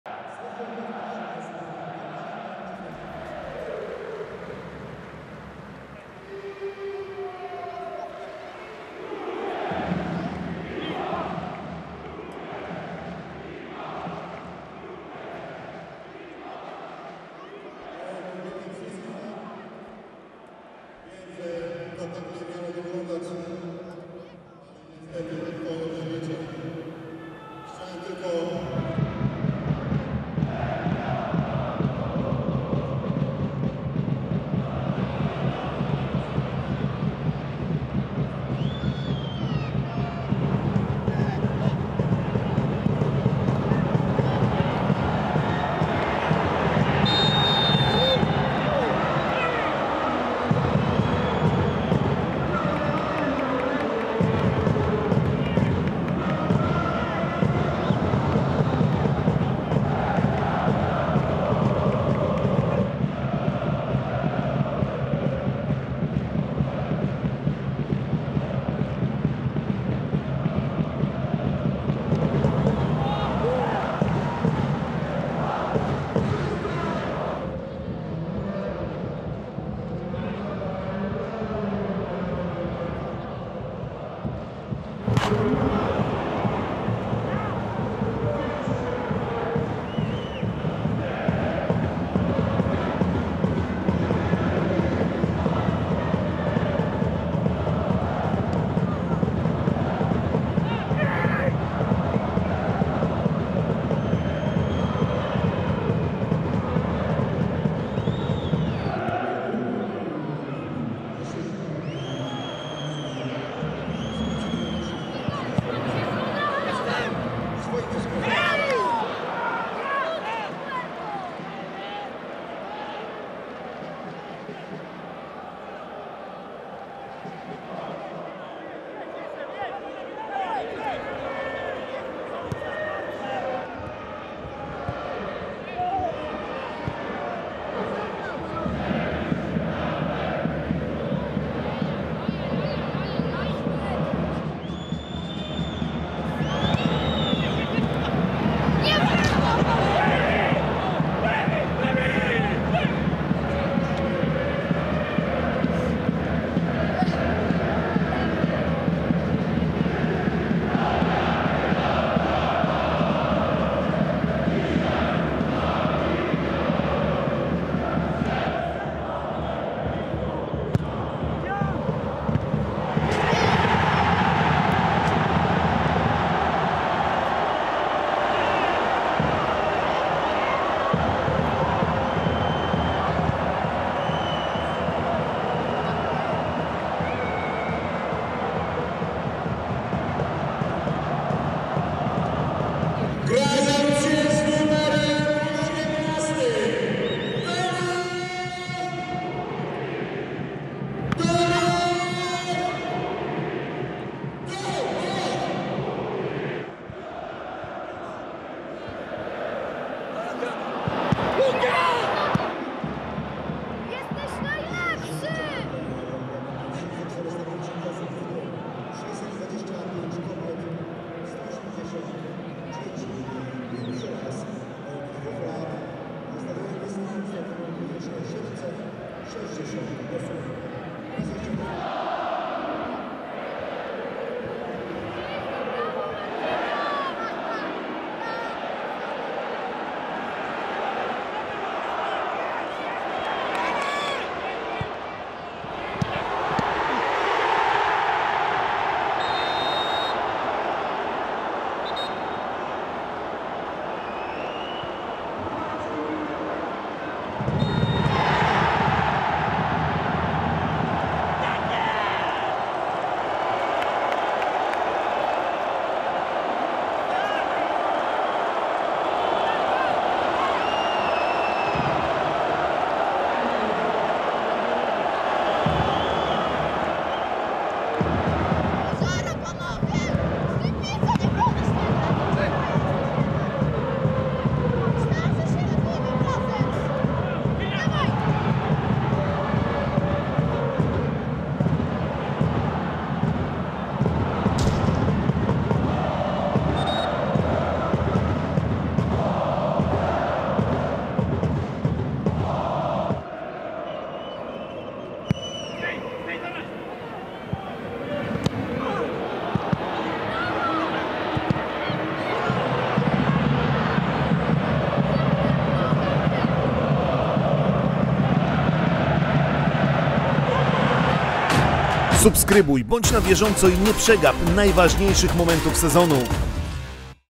“Jaga” lepsza od “Portowców”! | Jagiellonia - Pogoń | SKRÓT | Ekstraklasa | 2022/23 | 20. kolejka
W ostatnim sobotnim meczu 20. kolejki PKO BP Ekstraklasy Jagiellonia Białystok pokonała Pogoń Szczecin 2:0. Akcję spotkania gospodarze przeprowadzili pod koniec spotkania - Lewicki dośrodkował w pole karne, a Imaz uderzył z powietrza i Stipica musiał wyjmować drugi raz piłkę z siatki! Zwycięstwo dało “Jadze” awans na 12. miejsce w tabeli, Pogoń jest piąta.

Jagiellonia Białystok - Pogoń Szczecin 2:0
1:0 Tomasz Kupisz (58’)
2:0 Jesus Imaz (78’)

Zobacz Więcej!